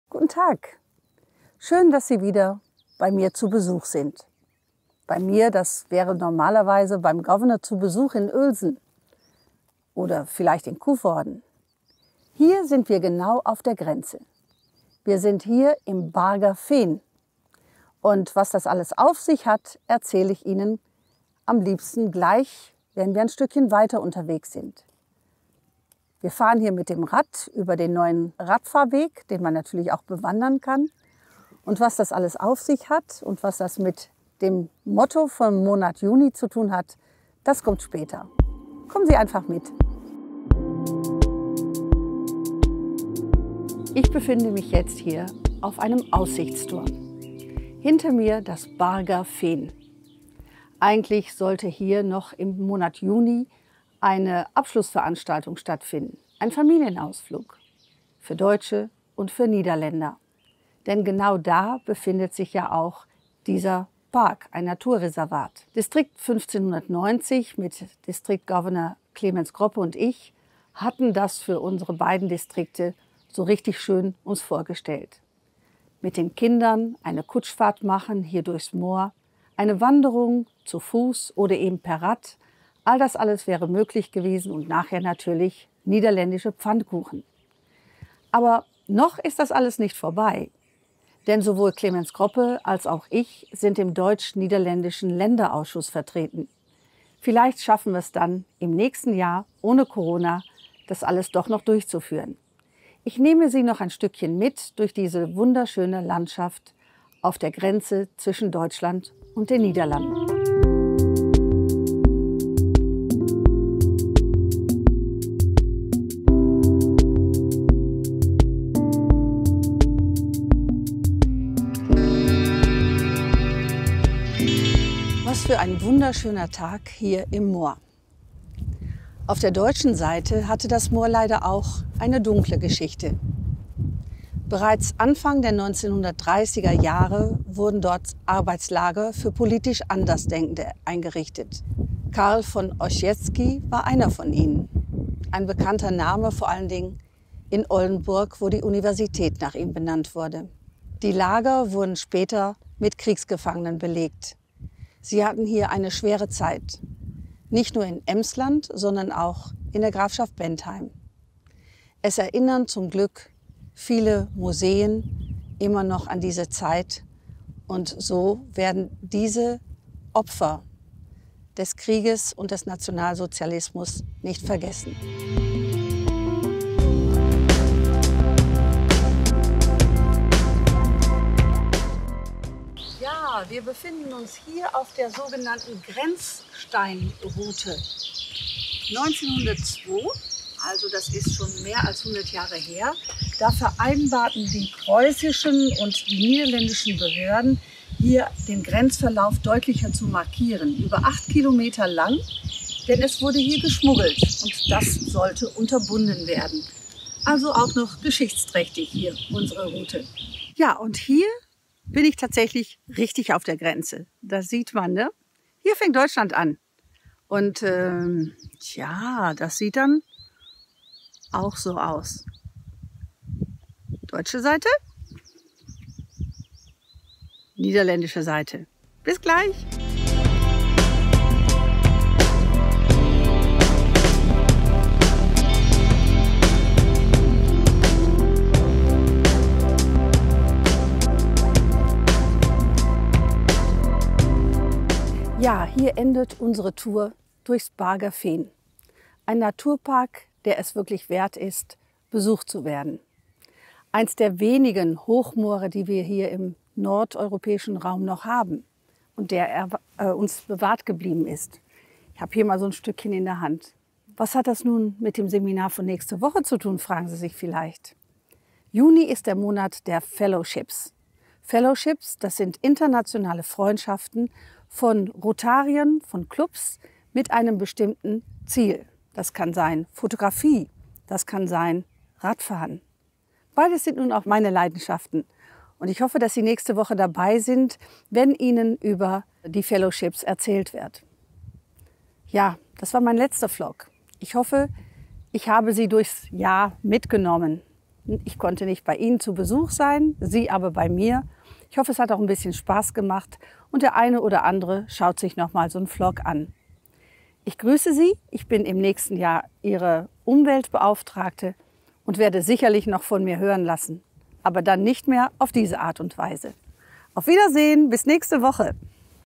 Der zwölfte Vlog von Governorin Marja Ritterfeld vom 9. Juni 2021
Governorin Marja Ritterfeld nimmt uns diesmal mit auf eine Radtour durchs deutsch-niederländische Grenzland. Und verrät, was sie nach ihrem Governorjahr machen wird.